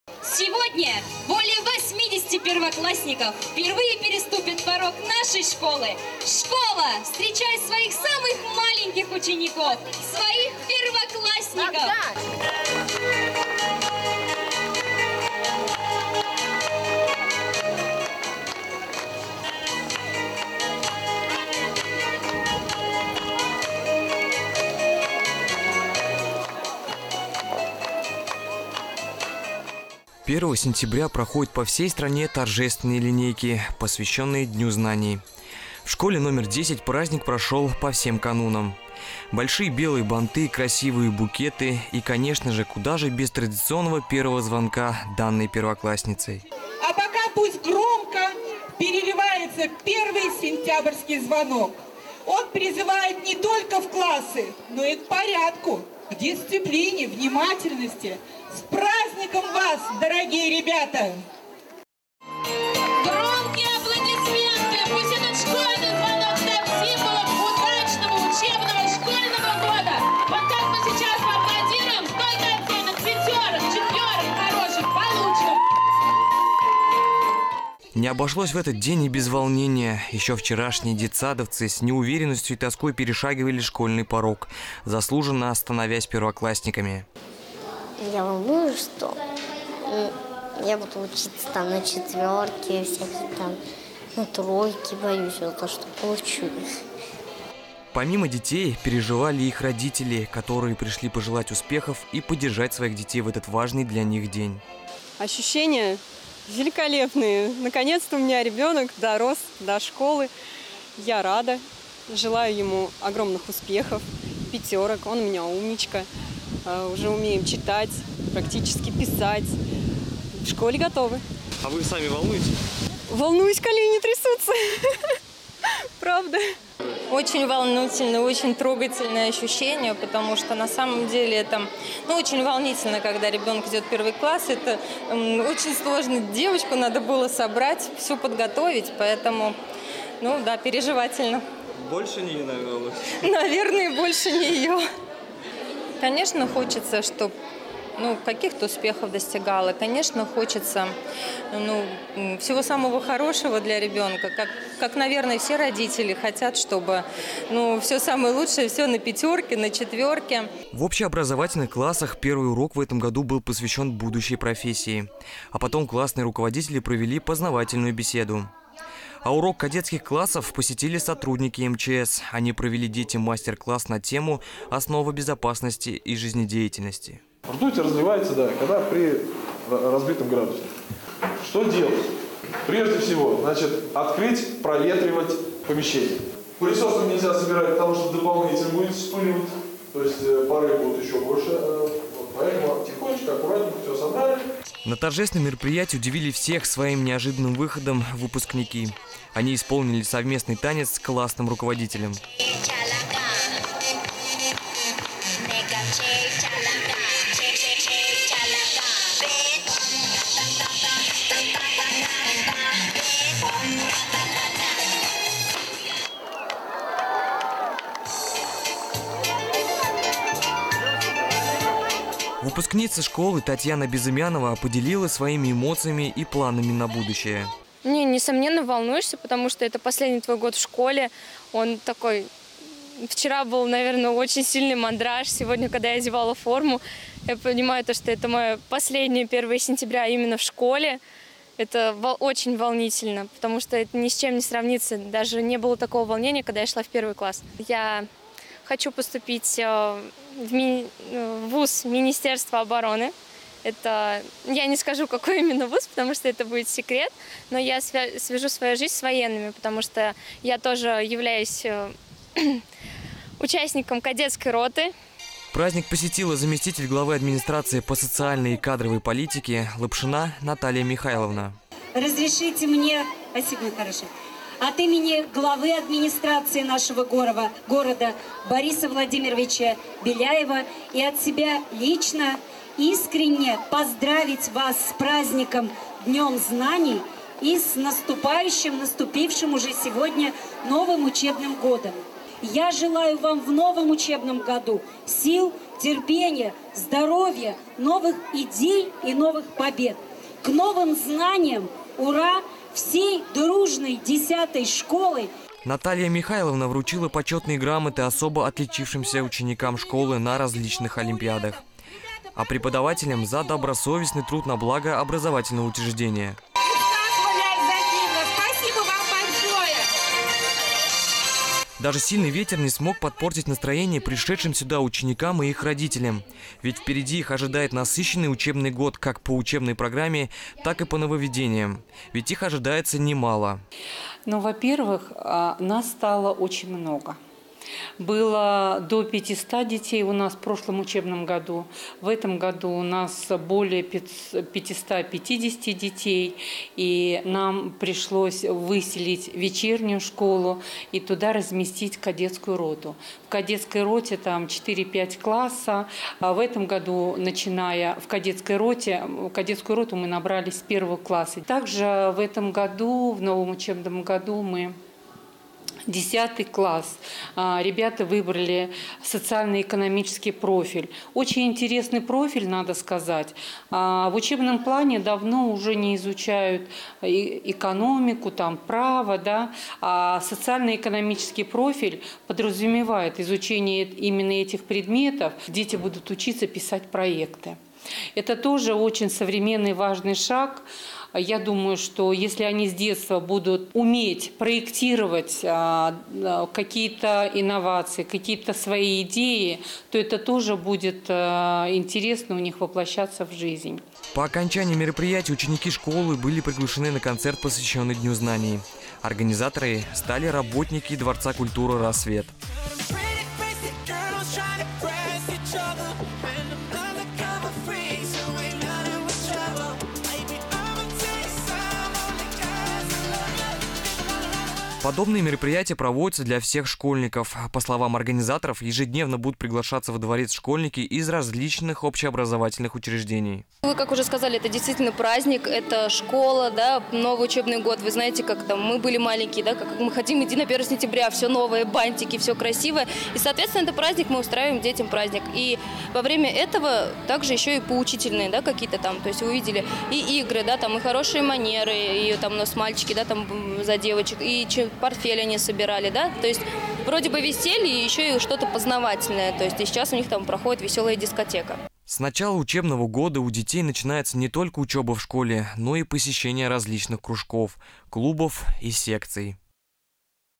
День знаний в школе №10
Летние каникулы миновали. Впереди новый учебный год! Сегодня 1 сентября и вся страна празднует день знаний. Кумертау не исключение . Как проходит торжественное мерпориятие в 10 школе узанавал корреспондент ТК Арис,